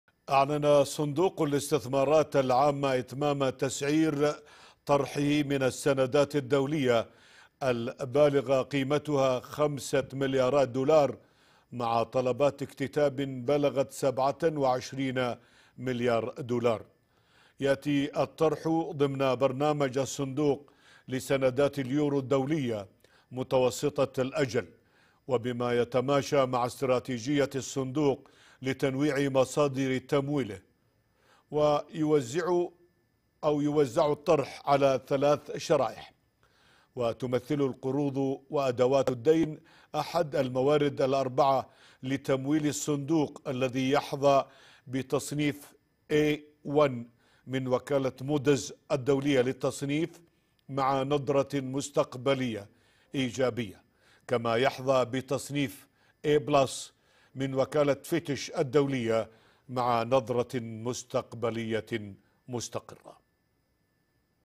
صندوق الاستثمارات يعلن إتمام تسعير طرح سندات بقيمة 5 مليارات دولار
أعلن صندوق الاستثمارات العامة إتمام تسعير طرحه من السندات الدولية البالغة قيمتها 5 مليارات دولار مع طلبات اكتتاب بلغت 27 مليار دولار.
ويأتي الطرح ضمن برنامج الصندوق لسندات اليورو الدولية متوسطة الأجل، وبما يتماشى مع استراتيجية الصندوق لتنويع مصادر تمويله، ويوزع الطرح على ثلاث شرائح ، وتمثل القروض وأدوات الدين إحدى الموارد الأربعة لتمويل الصندوق الذي يحظى بتصنيف A1 من وكالة موديز للتصنيف الائتماني مع نظرة مستقبلية إيجابية، كما يحظى بتصنيف A+ من وكالة فيتش مع نظرة مستقبلية مستقرة.